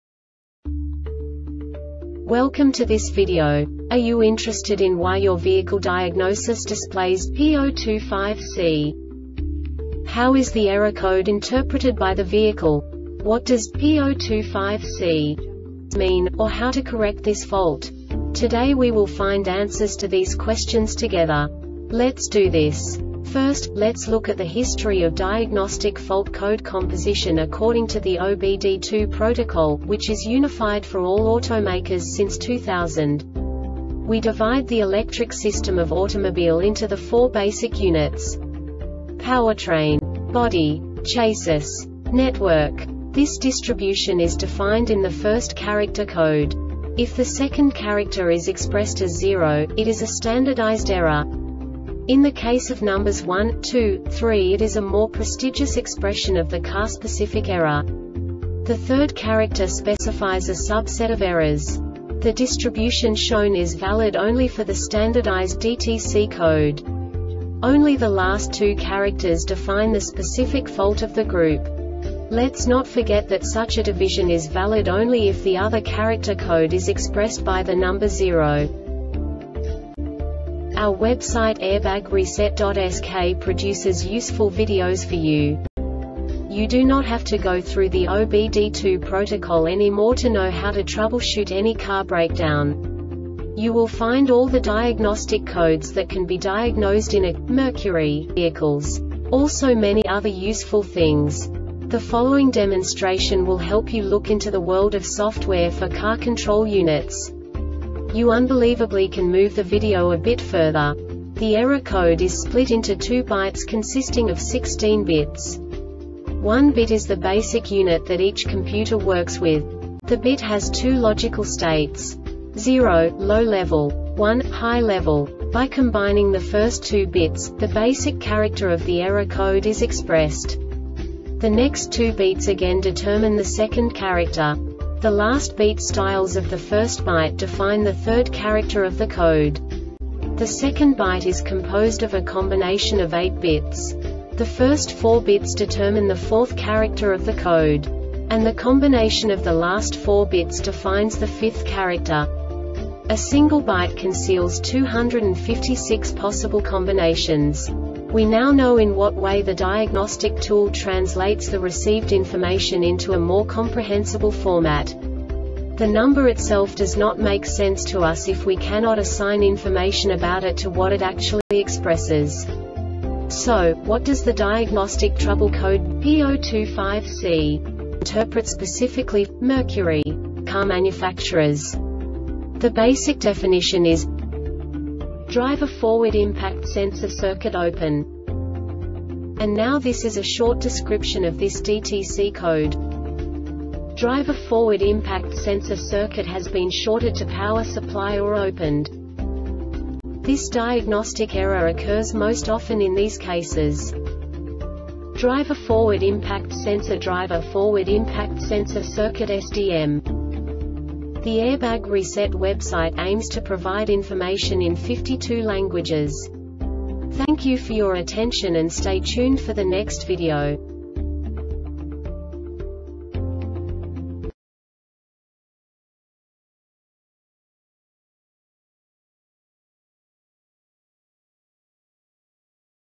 DTC Mercury P025C Short Explanation
Contents:
0:21 Basic DTC analysis according to OBD2 protocol standard.
1:48 Insight into programming
2:58 A diagnostically understandable description of the Diagnostic Trouble Code
3:05 Basic error definition

Make:
Mercury
Code:
P025C
Definition:
Driver Forward Impact Sensor Circuit Open
Description:
Driver forward impact sensor circuit has been shorted to power supply or opened.
Cause:
Driver forward impact sensorDriver forward impact sensor circuitSDM